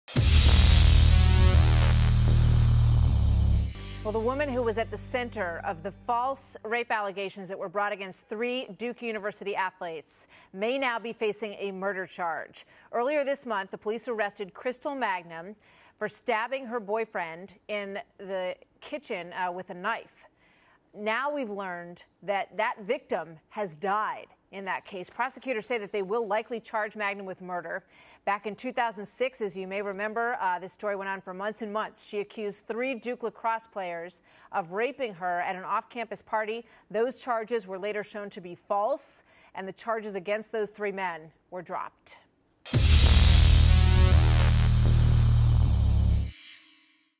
False Accuser of Duke Lacrosse Players May Face Murder Charge
The woman who falsely accused Duke lacrosse players of rape may be facing seriousl charges of her own. Martha MacCallum has the full story.